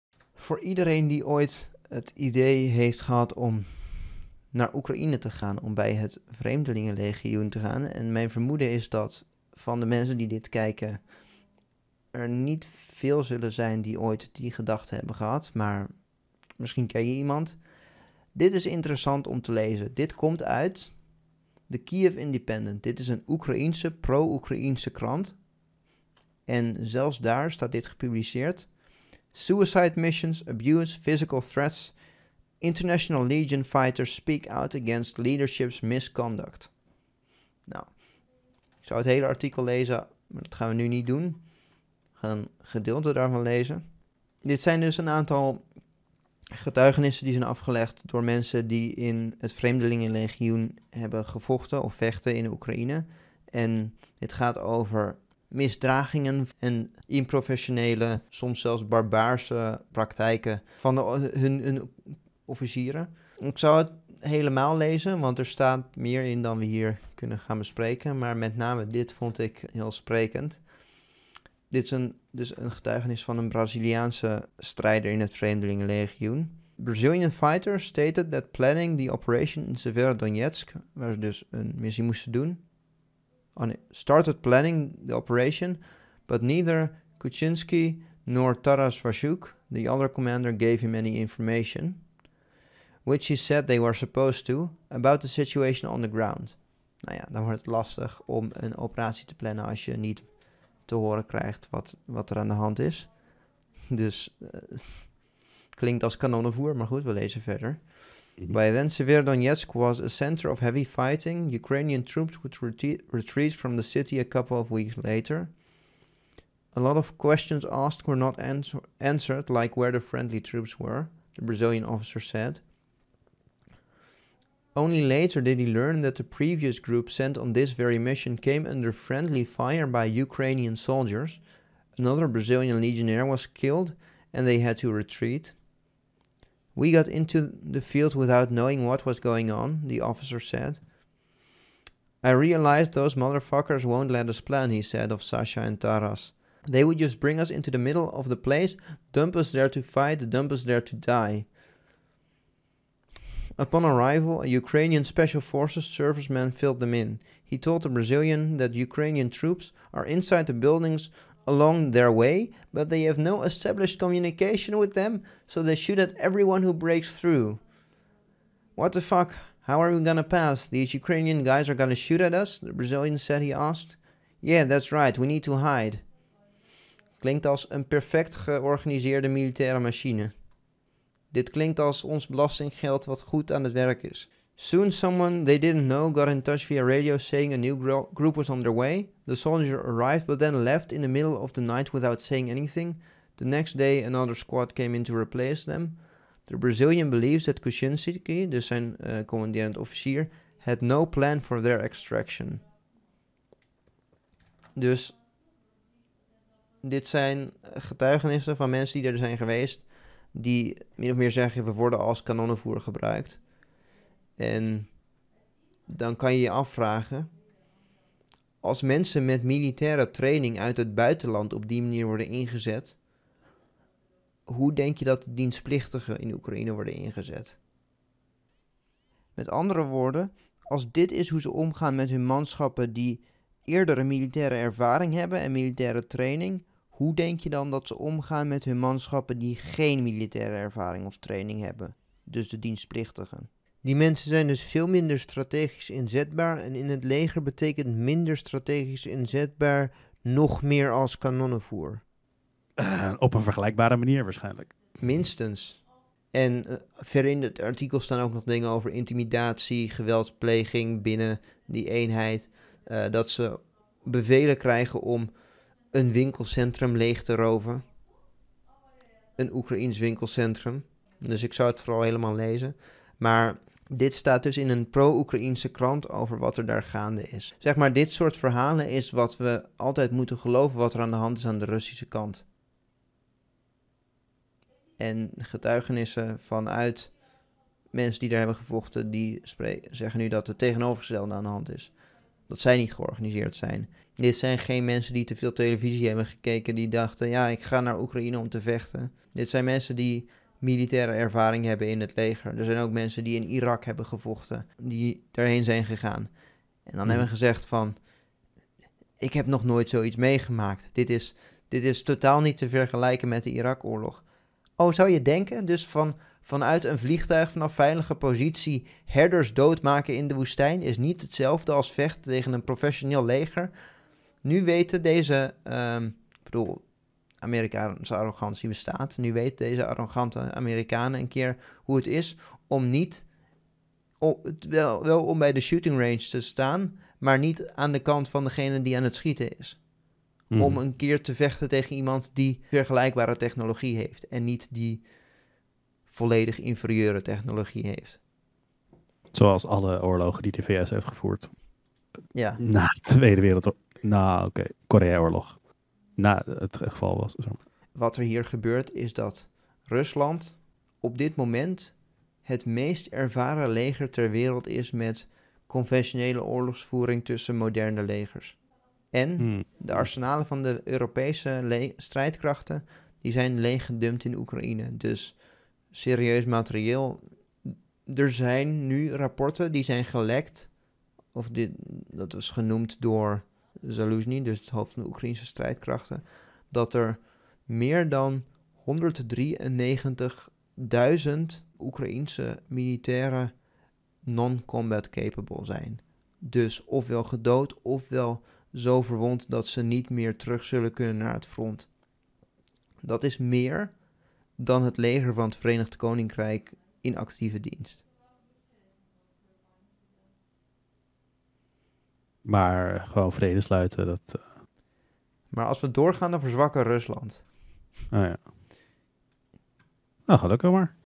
GETUIGENIS: Oekraïense Troepen Gebruikt als KANONNENVOER
Een reeks getuigenissen uit het Oekraïense vreemdelingenlegioen, gepubliceerd in de pro-Oekraïense Kyiv Independent, laten zien dat het Oekraïense leger hun strijders als kanonnenvoer.

Gebruikte artikelen / videos op volgorde

The Kyiv Independent - Suicide missions, abuse, physical threats: International Legion fighters speak out against leadership’s misconduct